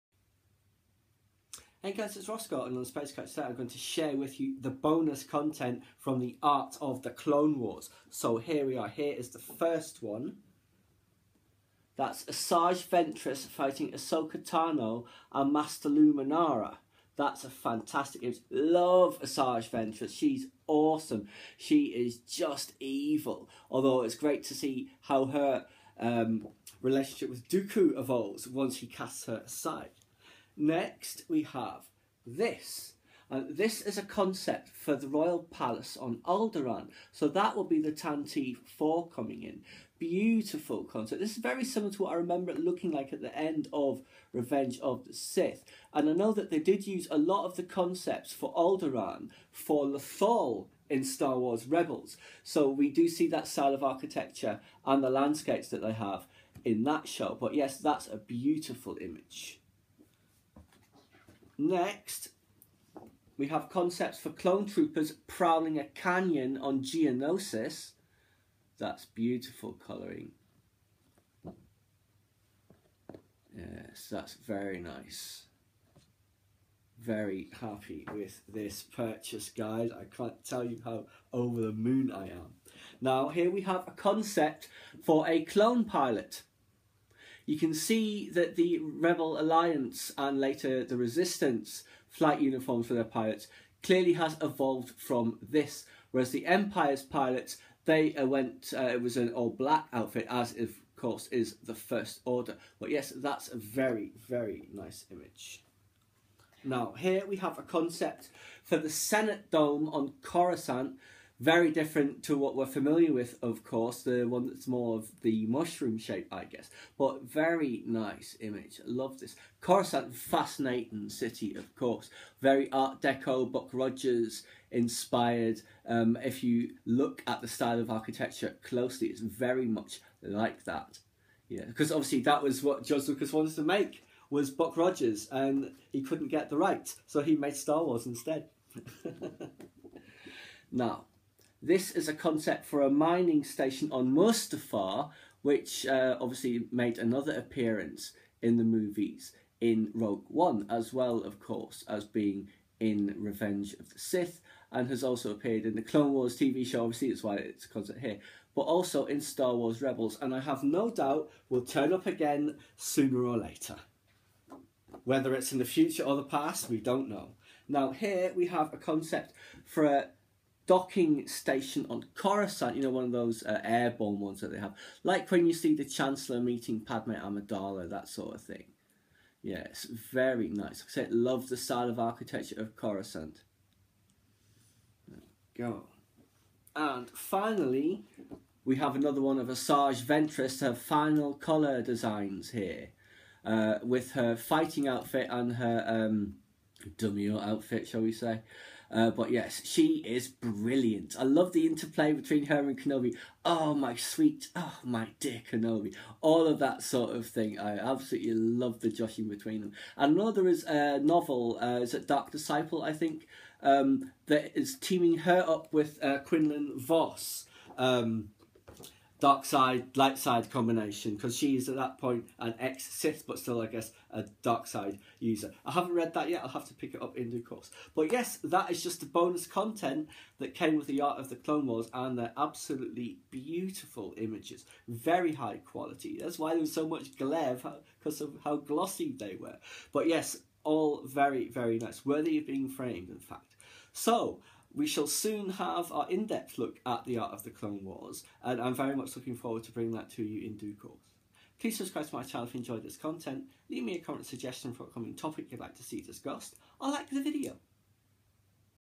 The Art of The Clone Wars bonus content!
Some glorious, high-quality prints were included with the Clone Wars art book...and where I say Buck Rodgers, I meant Flash Gordon 😂😂😂😂